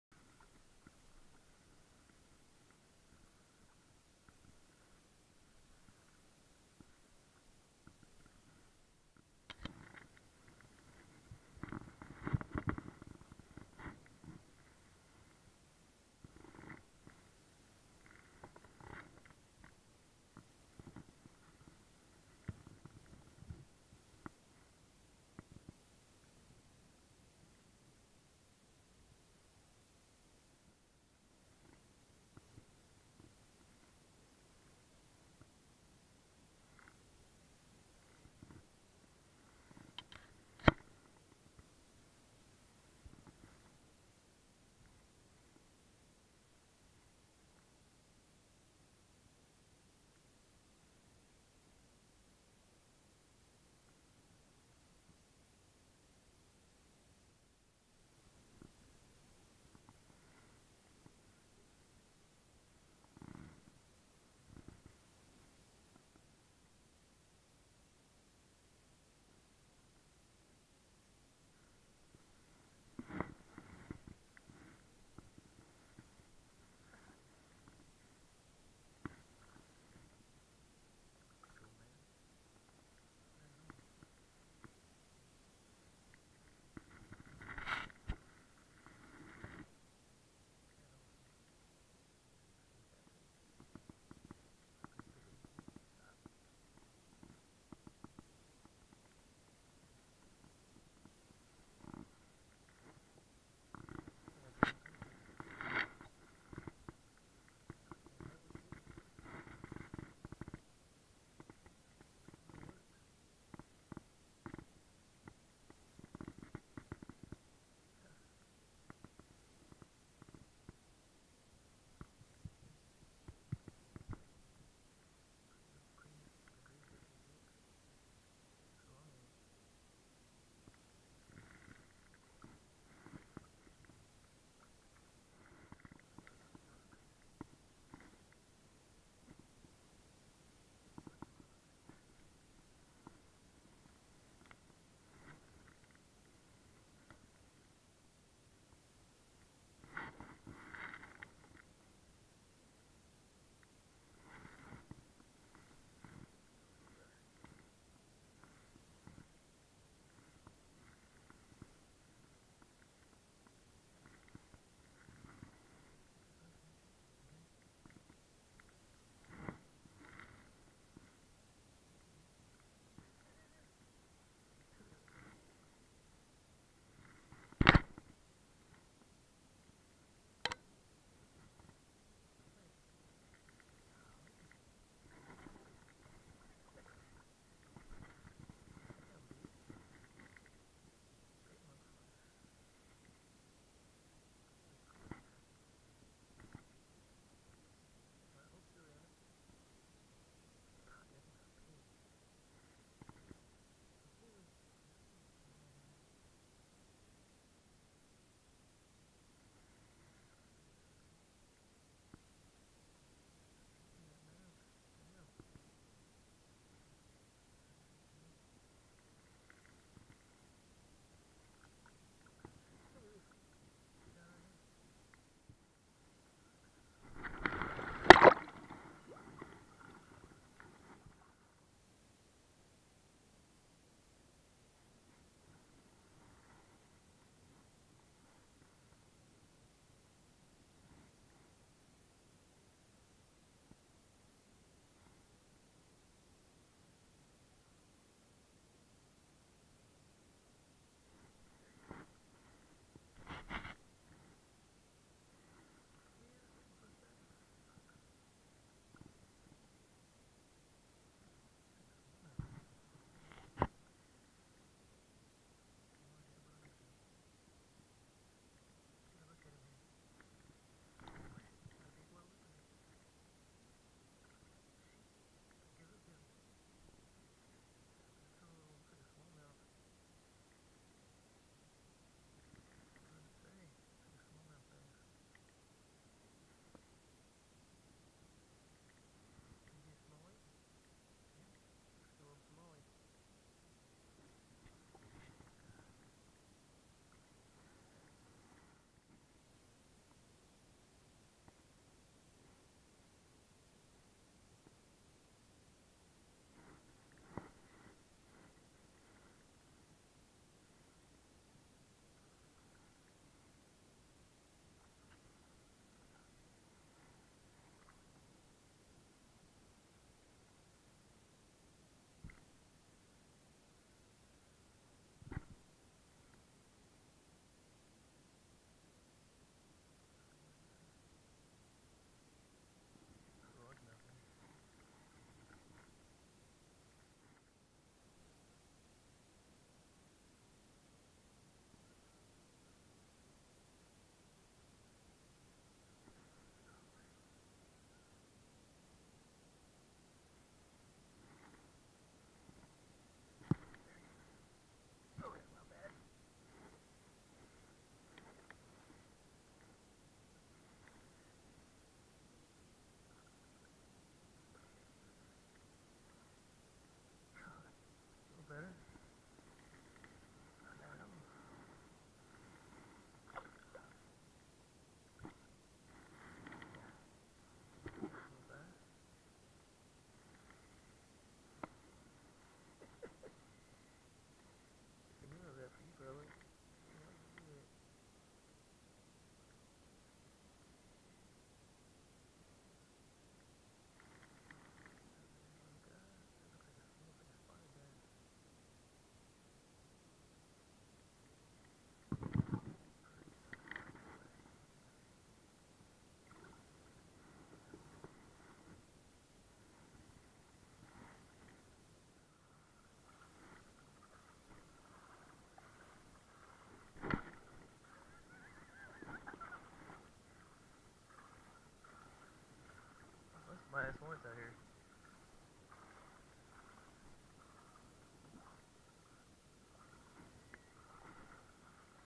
Fly fishing the Guadalupe River 8 14 16
I can confirm that last bass was just a largemouth bass and not a Guadalupe bass. Maybe next time!